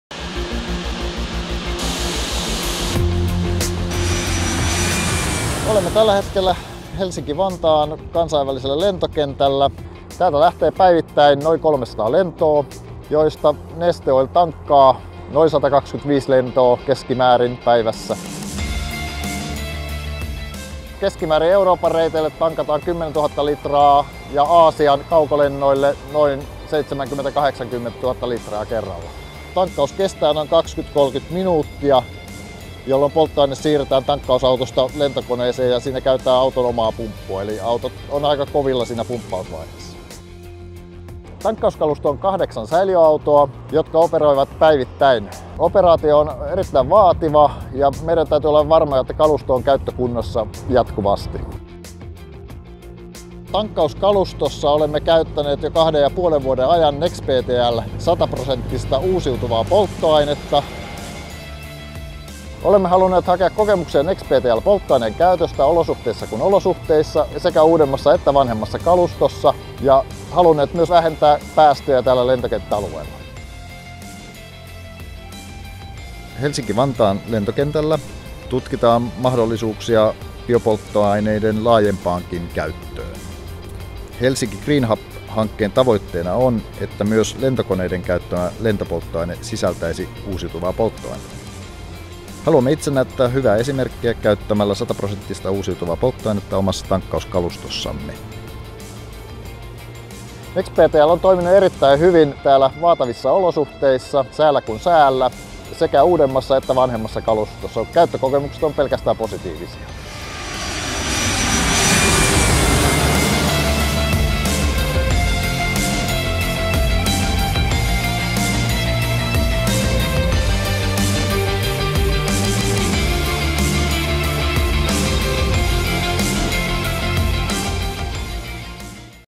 Neste Green Hub project
Use of renewable diesel promotes Helsinki Airport's Green Hub project